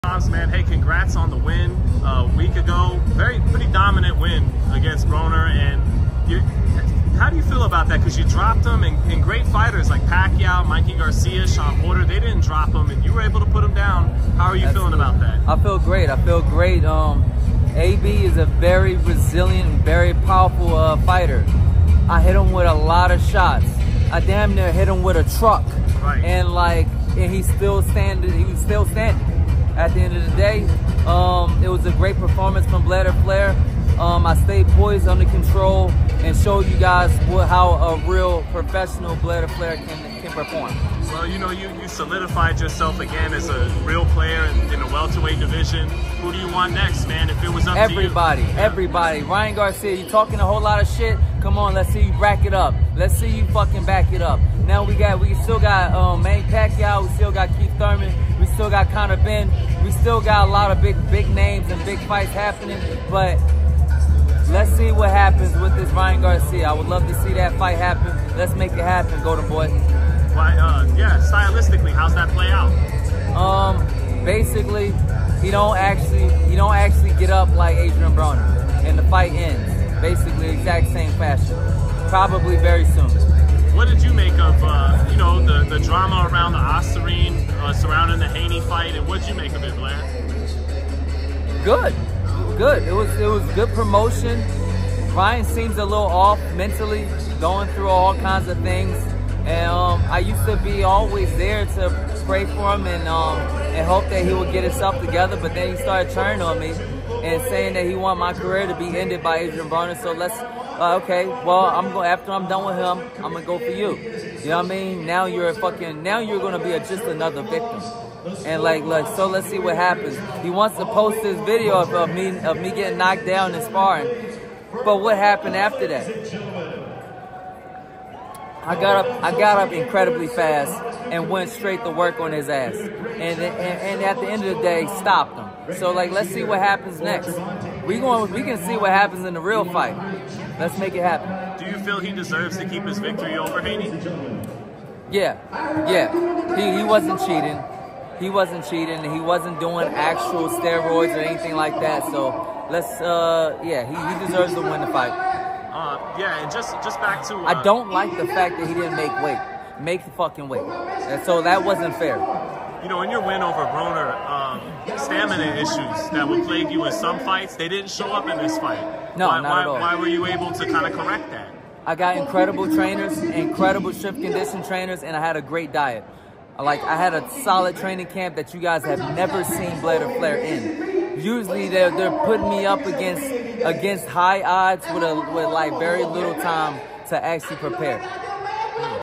BLAIR COBBS CHECKS RYAN GARCIA ON SPARRING KNOCKDOWN; SAYS "HE DOESN'T GET UP LIKE BRONER DID."
Coming off his big win over Adrien Broner, Blair Cobbs (17-1-1, 10 KOs), calls out Ryan Garcia (25-1, 20 KOs) for a big fight at 147. Cobbs also addresses the leaked sparring tape of the two and wants to settle it in a real fight in the ring.

Interview by Sean Zittel.